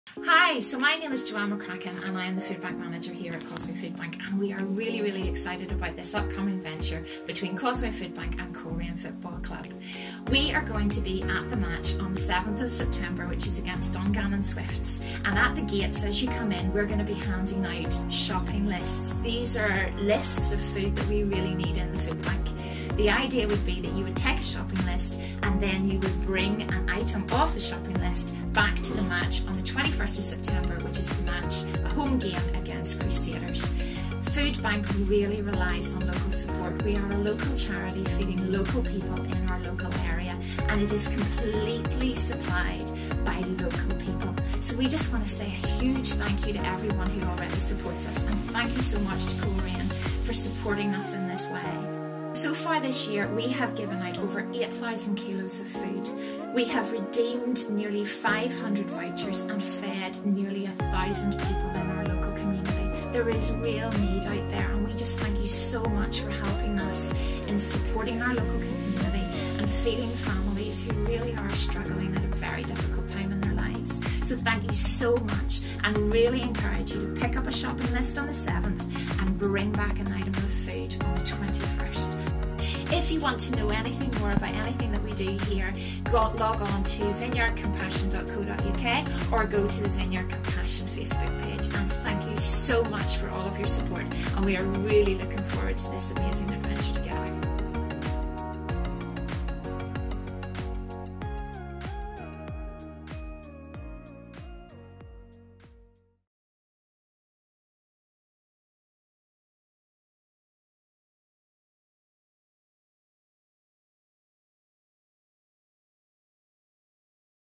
COMMUNITY | Coleraine FC supports Causeway Foodbank | Wednesday 4th September 2019
If you are heading our game against Dungannon Swifts on the 7th of September, be sure to grab a shopping list and help those in need